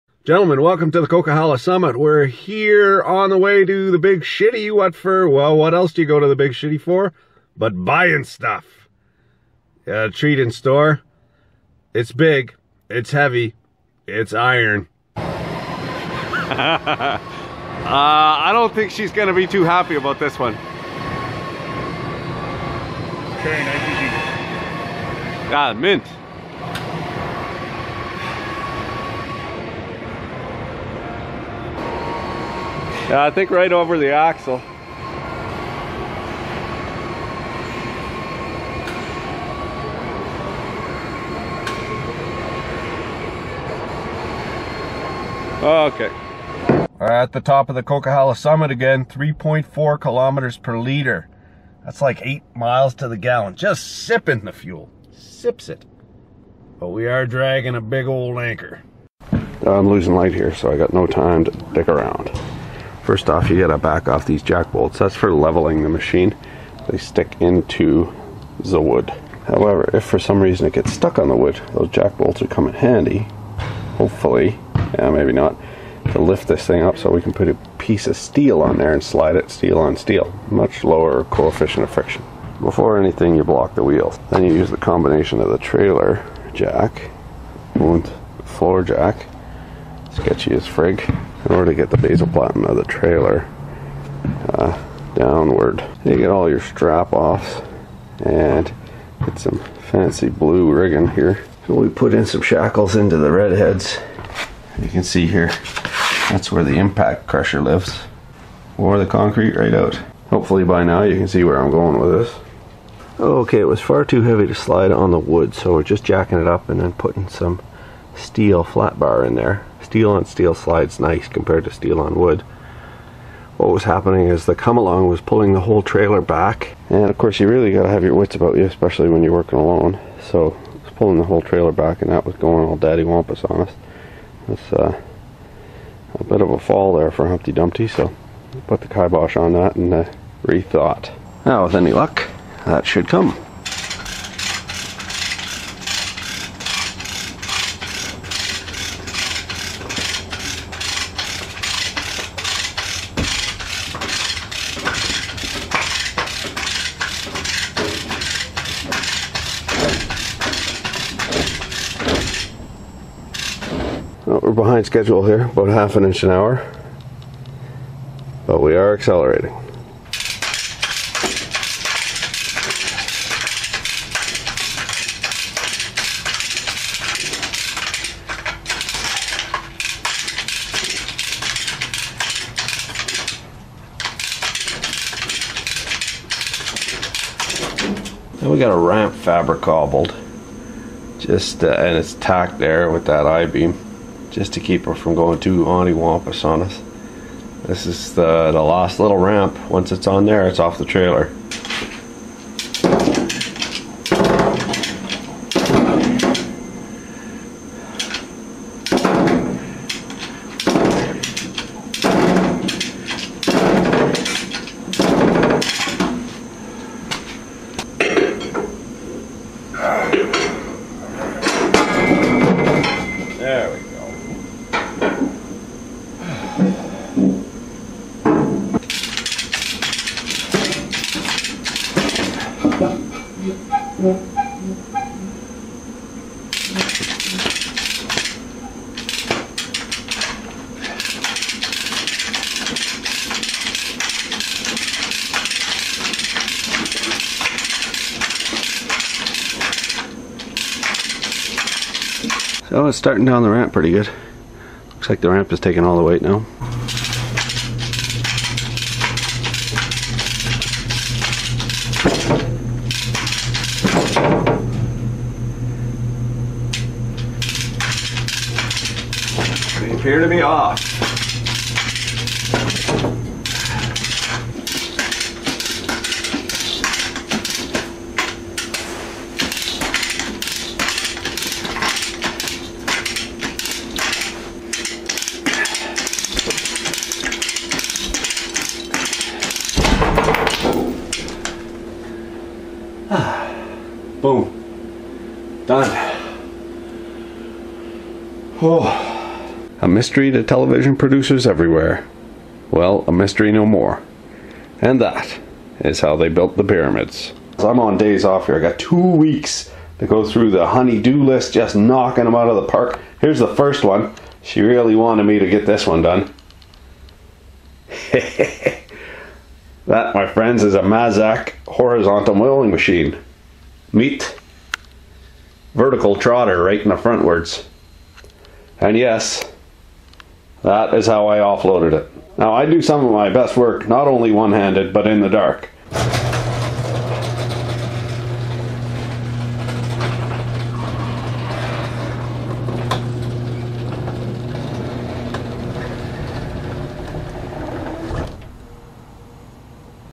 How to move heavy iron SOLO. Cheap, Dirty and Downright Dangerous.
"Everybody wants to be a bodybuilder but nobody wants to lift no heavy ass weights." -Ronnie Coleman
This is the poorman's way to sling heavy iron around. No friends to help? No budget for cranes? No problem! The Egyptians built the pyramids, surely we can get a 5000LBS machine into the shop. Wear you safety squints and watch your toes, this is gonna get sketchy!
If you'd like to be a part of making vids please consider tossing a coin towards the channel's long term projects